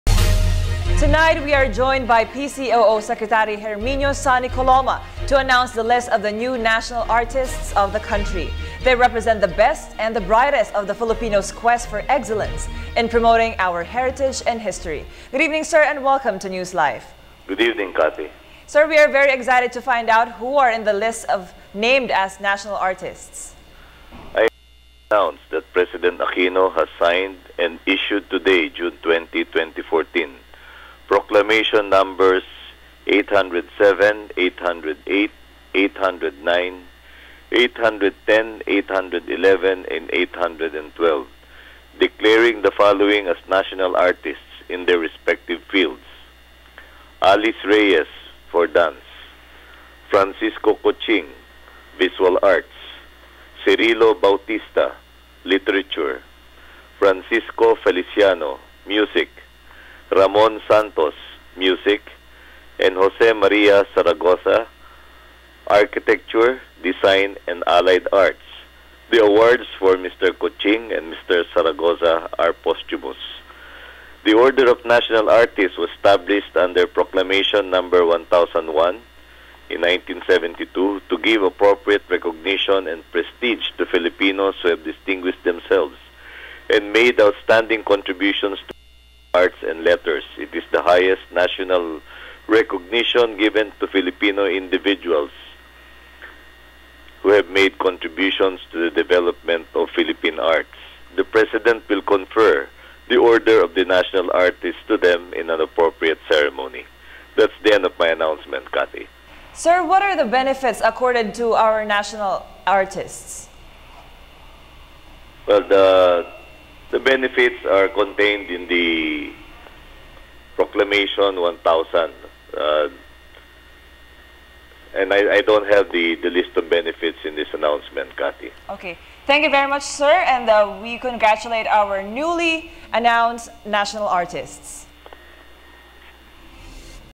NewsLife Interview: Sec. Sonny Coloma, PCOO - on 'List of new PHL national artists' [6/20/14]
NewsLife Interview: Sec. Sonny Coloma, PCOO - on 'List of new PHL national artists' - [June 20, 2014]